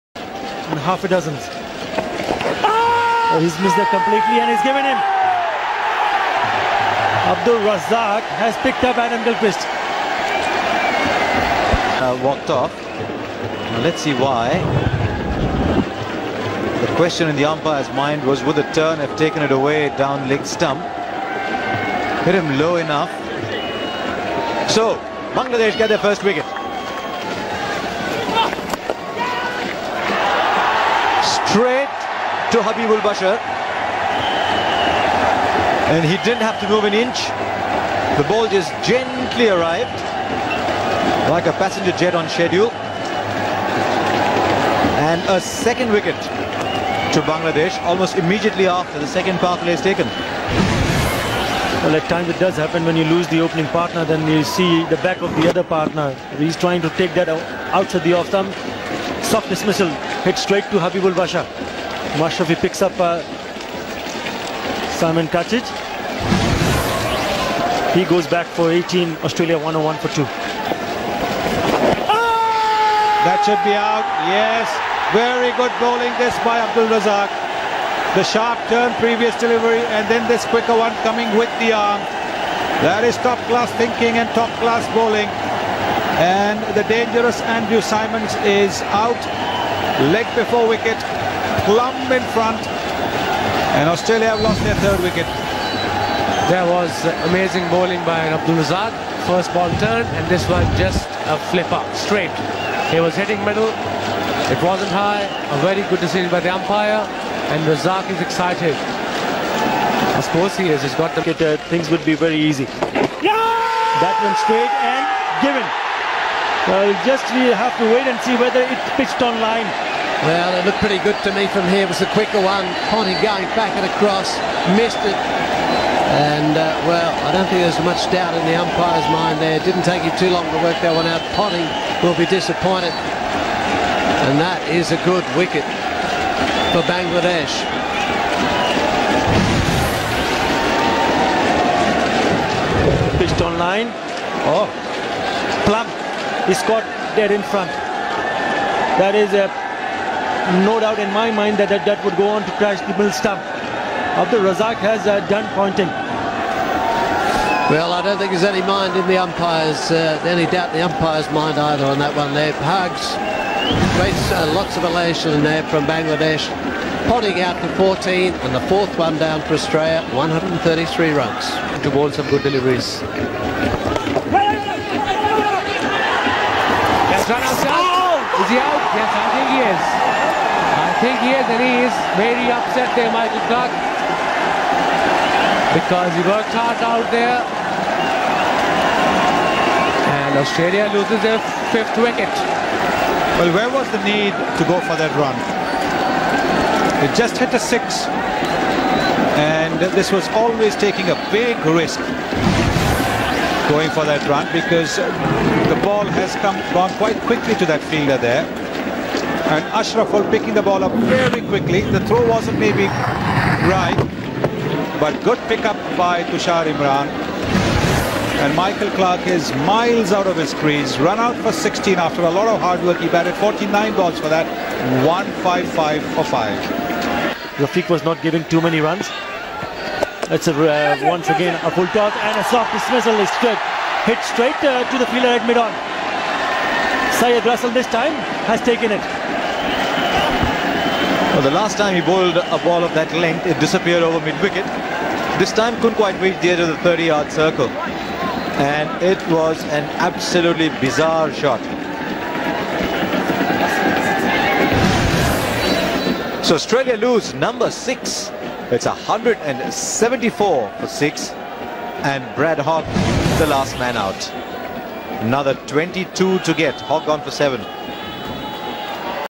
HABIBUL BASHAR 70 against Australia Cricket highlights
HABIBUL BASHAR 70 (114) --- OLD is GOLD __ vs AUSTRALIA 2nd ODI 2006 __ MUST WATC